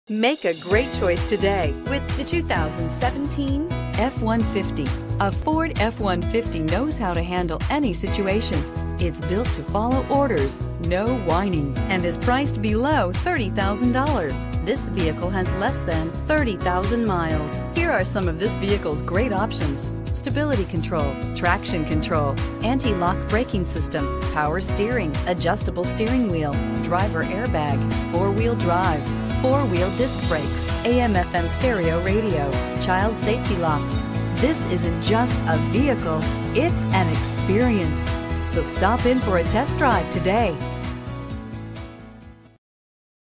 2017 Ford F-150 RM1980
2017 FORD F-150 Grafton, WV
Stock# RM1980

Toothman Ford
Us Route 50 West

Looking for the right car? Today could be your lucky day. With 28383 miles, this Silver, 2017 Ford F-150 equipped with Automatic transmission could be yours. Request more information and set up a test drive right away.